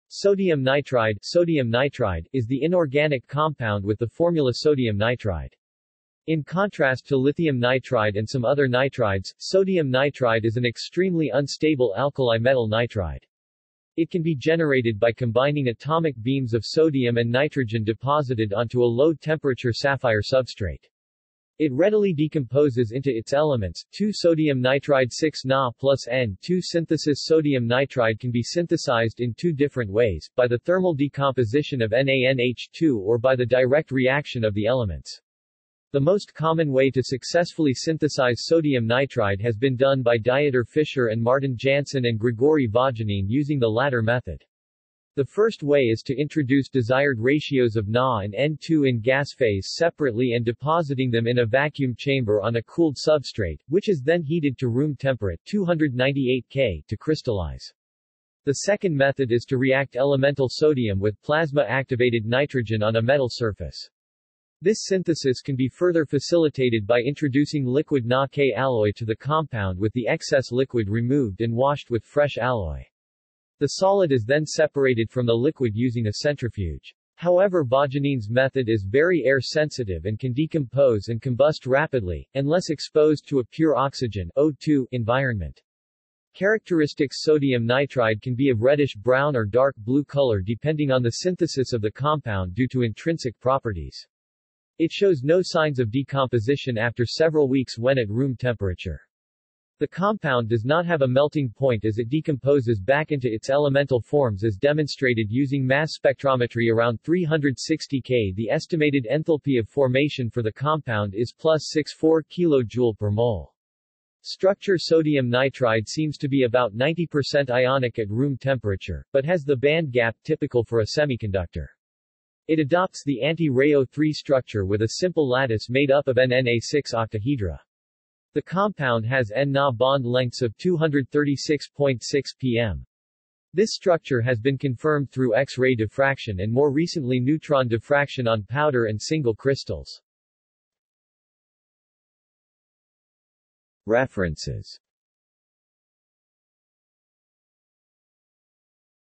Sodium nitride | Wikipedia audio article
This is an audio version of the Wikipedia Article:
Sodium nitride

SUMMARY
Sodium nitride (Na3N) is the inorganic compound with the formula Na3N.  In contrast to lithium nitride and some other nitrides, sodium nitride is an extremely unstable alkali metal nitride. It can be generated by combining atomic beams of sodium and nitrogen deposited onto a low-temperature sapphire substrate.
It readily decomposes into its elements:

2 Na3N → 6 Na + N2